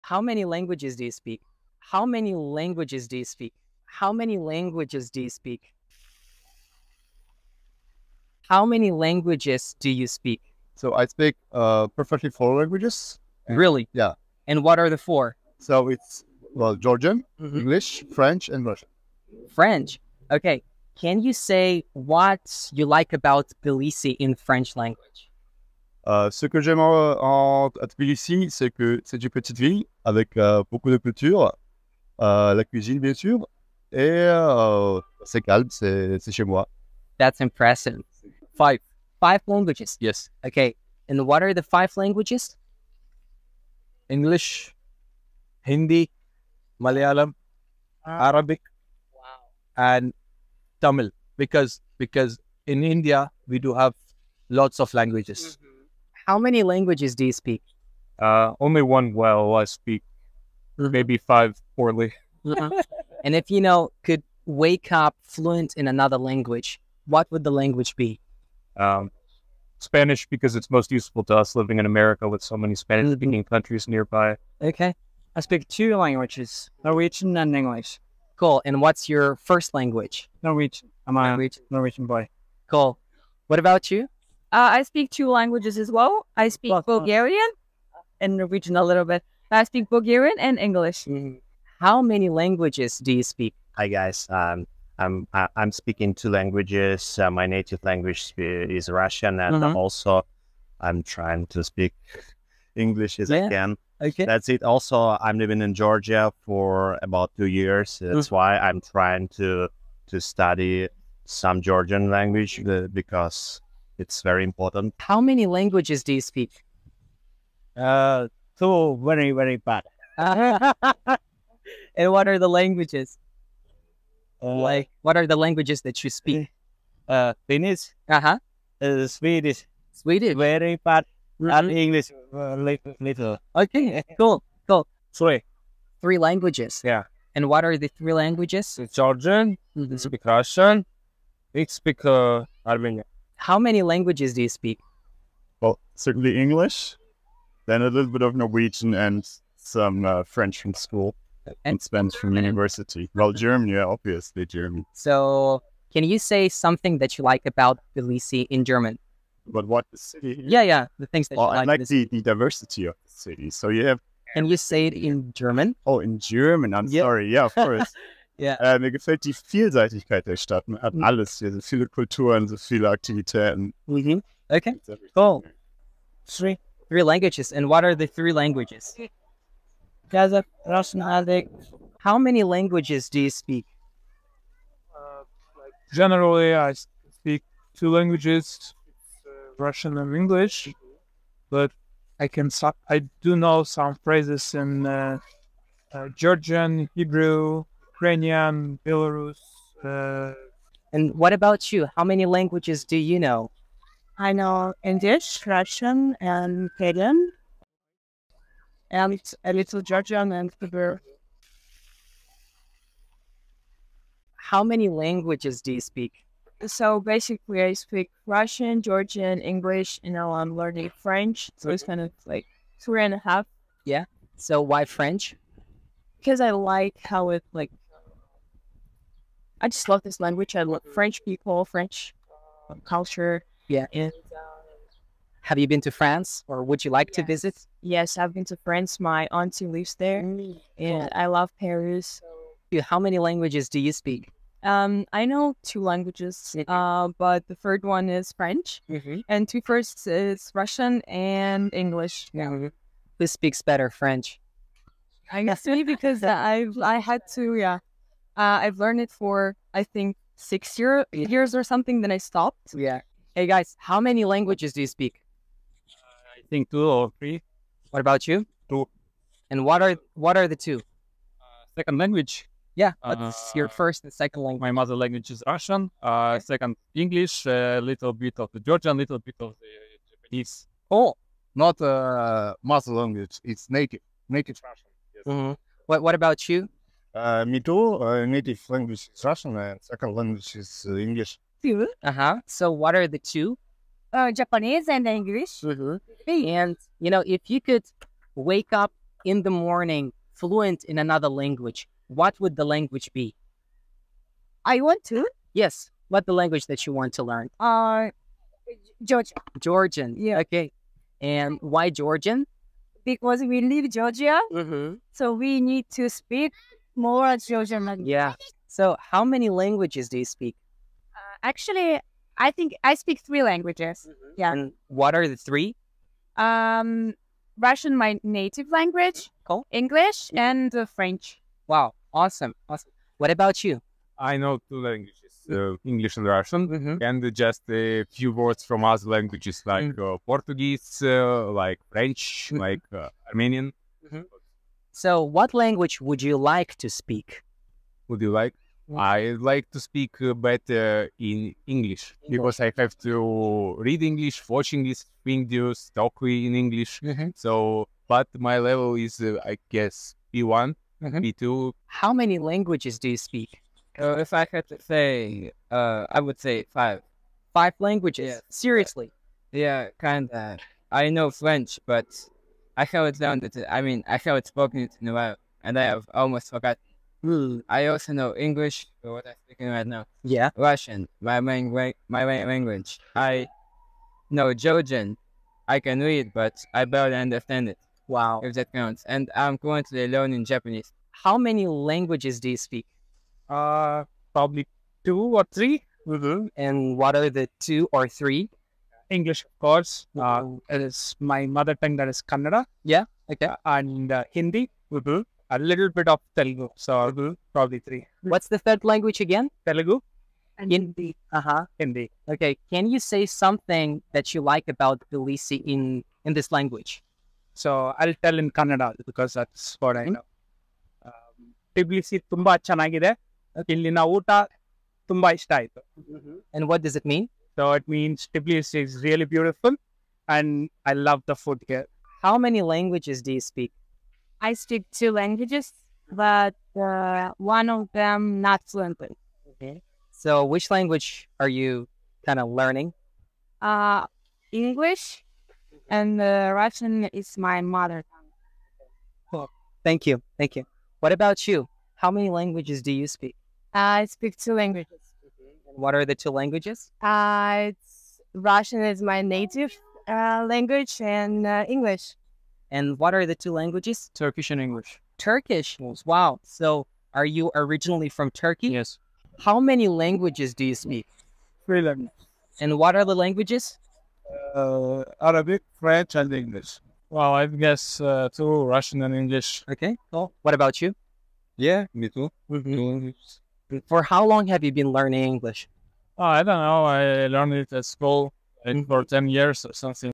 How many languages do you speak? (Tbilisi, Georgia)
Curious about the linguistic richness of Tbilisi?

Join us in this engaging YouTube video as we hit the streets to explore the multilingual talents of the city's residents. From Georgian to international languages, watch as we chat with locals and foreigners about their linguistic abilities in Tbilisi.

Special thanks to Alex Korolev, the camera man!